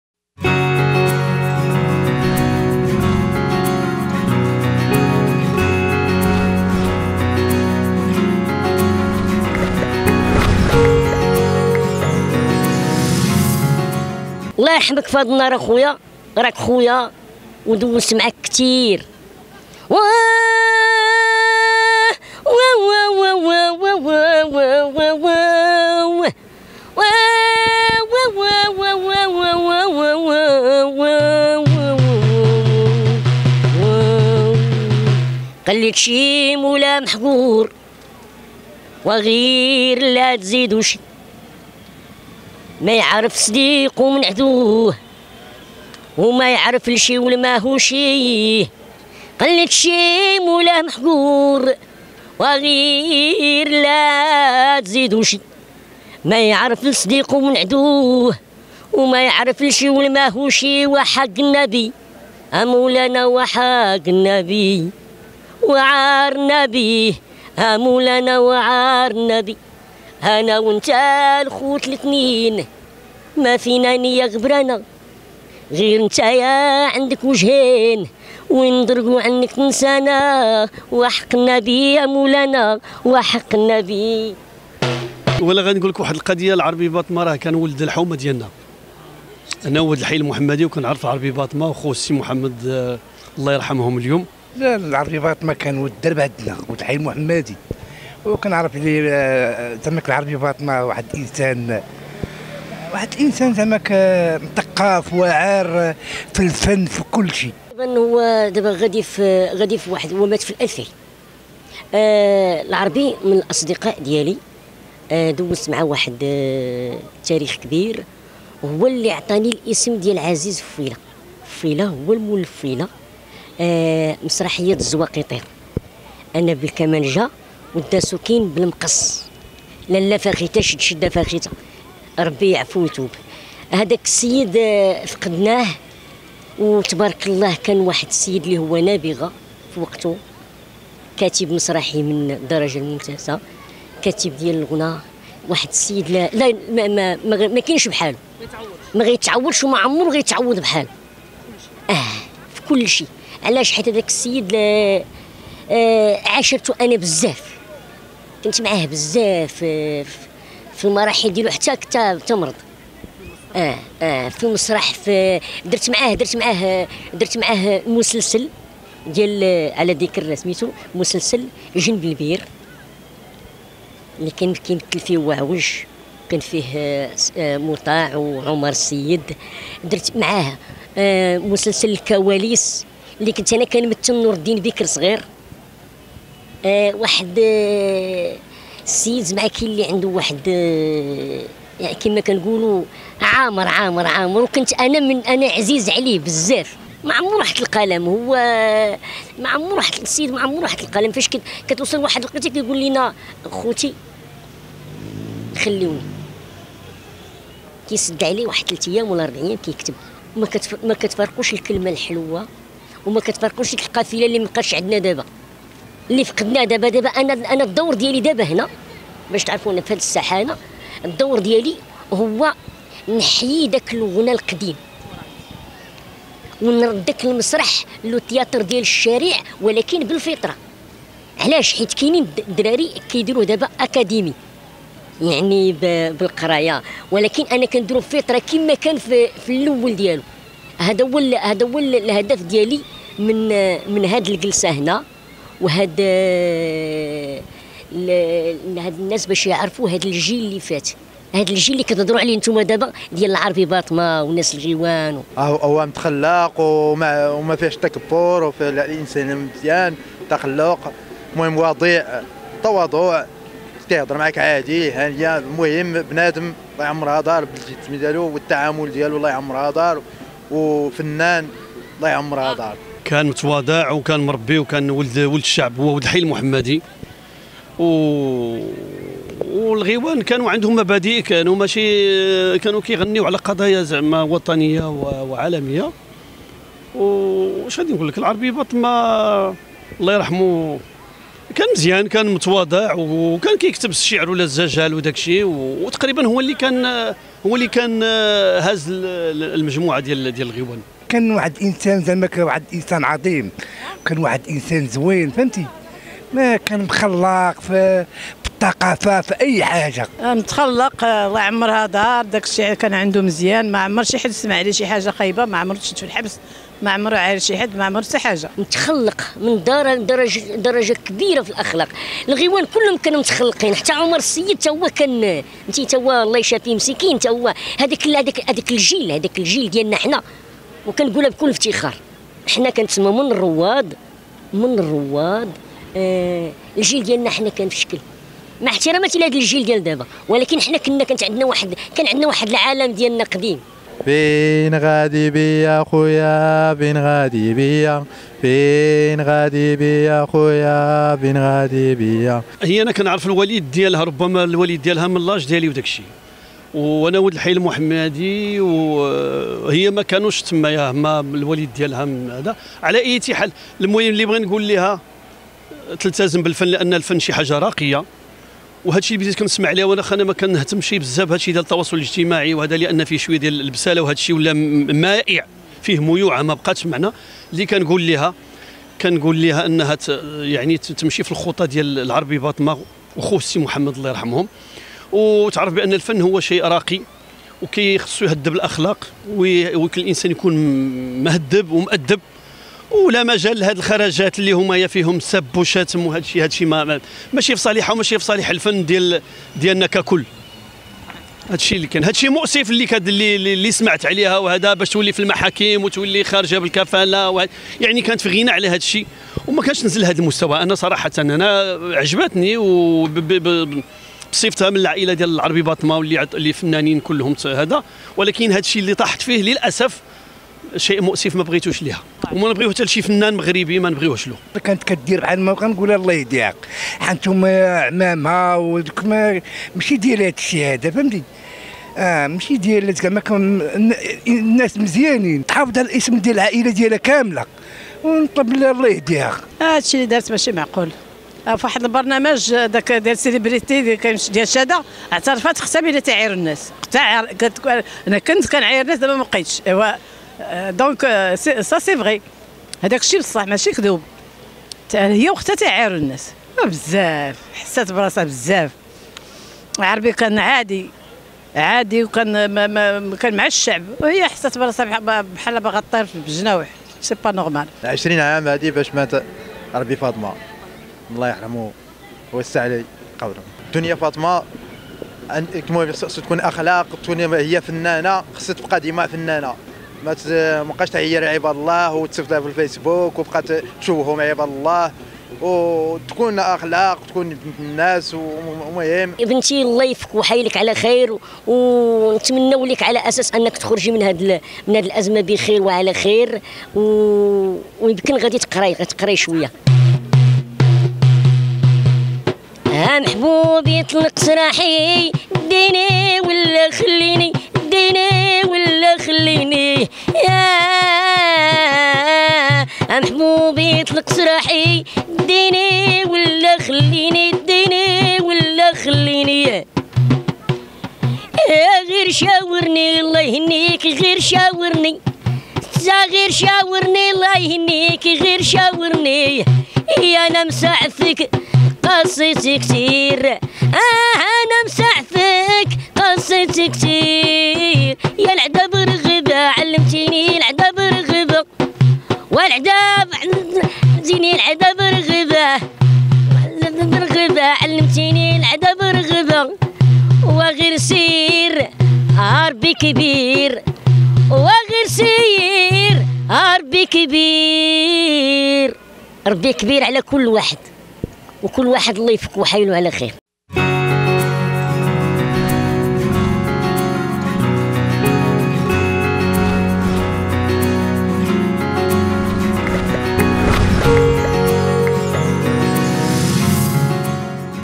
في الذكرى 22 لوفاة العربي بطمة..مغاربة يقصفون دنيا بطمة :"شوهات العائلة والعربي كان مثقف ومربي"
في الذكرى 22 لوفاة الأسطورة الغيوانية العربي بطمة، قصف مغاربة دنيا بطمة بعد فضيحة حساب حمزة مون بيبي.